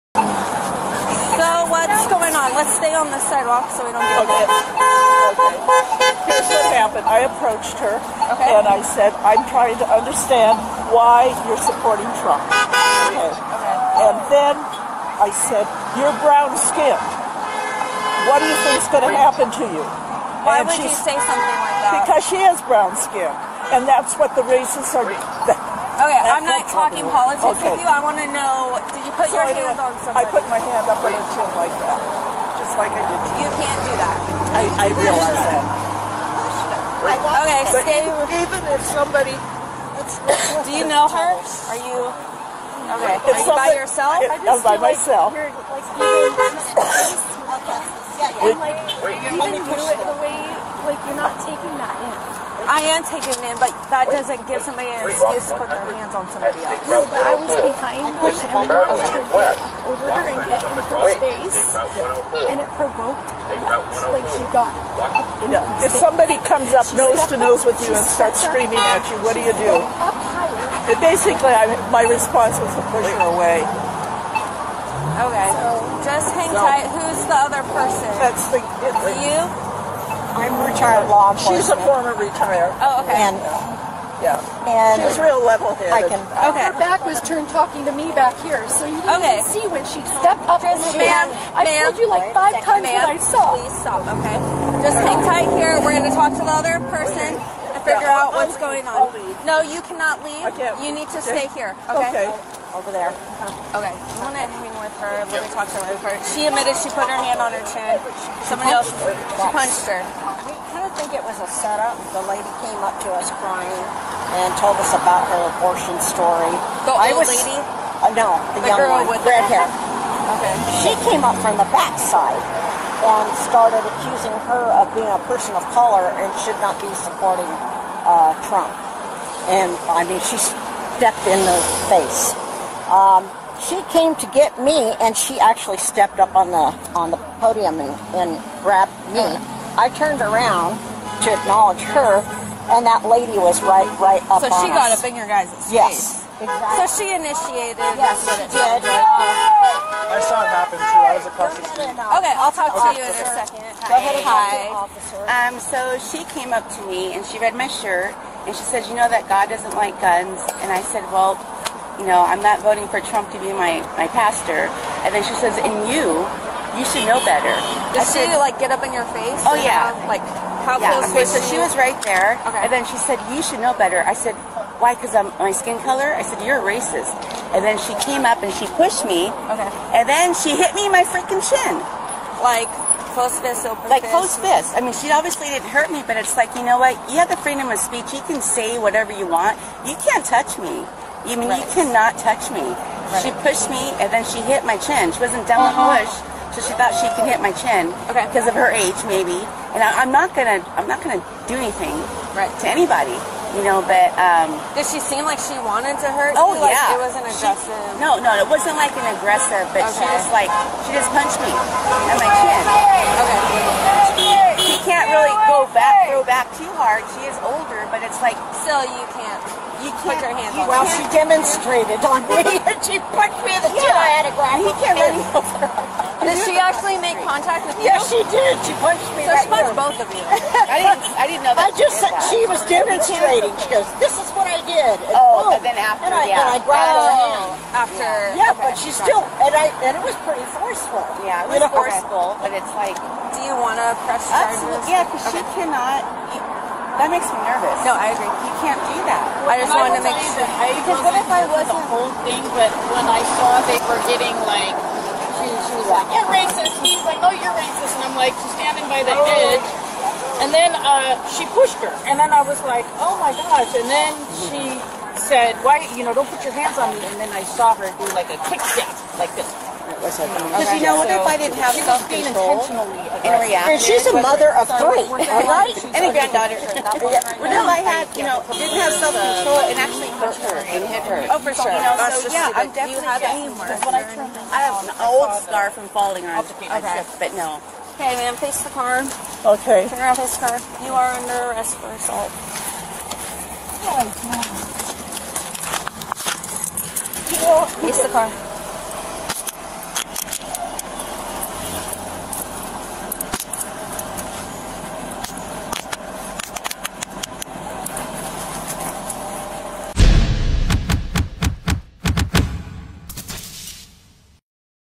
TDS makes people feel they are entitled to assault someone who doesn't share their political views.
It's beyond insanity.
I wonder if the disingenuous media and the Democrat elitists are proud of the division they created.

“You can’t do that!” Police bodycam footage captured the aftermath of an 82-year-old woman being arrested after allegedly assaulting a Trump supporter who she confronted and lec